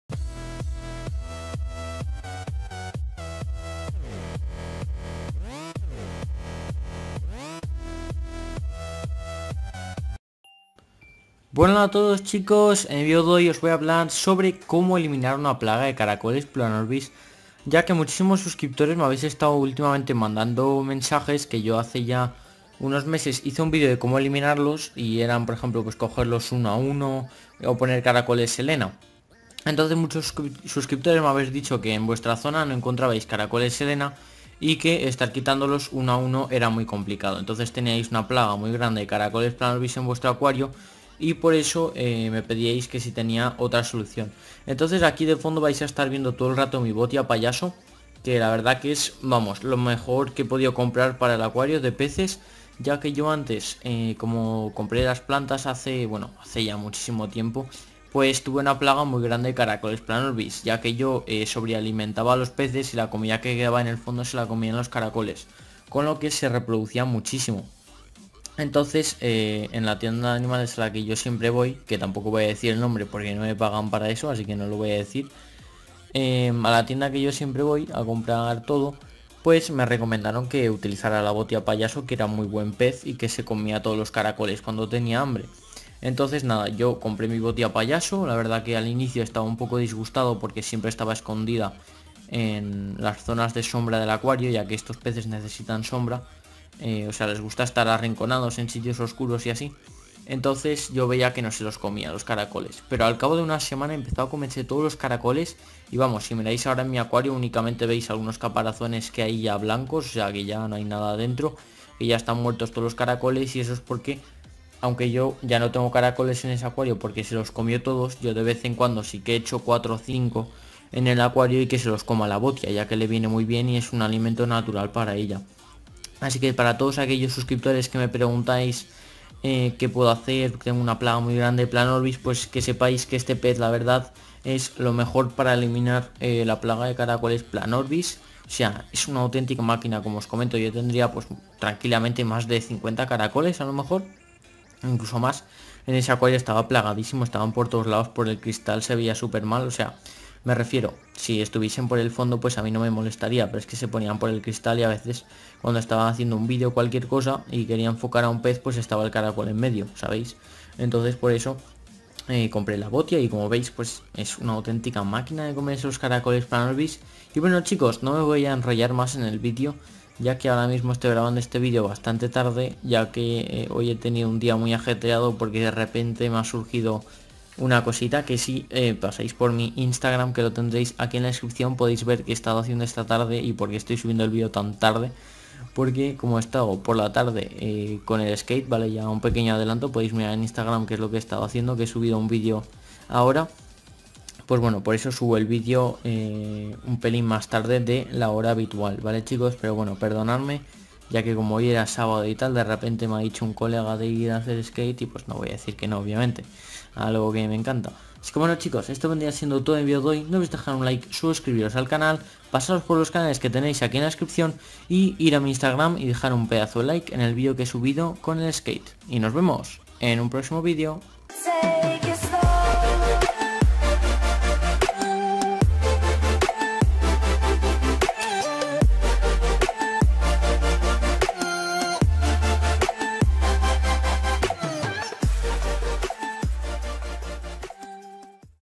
El Mejor Pez para Eliminar Plaga de Caracoles Planorbis, la Botia Payaso | Rincon Animal
Mis otros canales:

El Rincon De Pedro

Pedropedrola

GamesOnline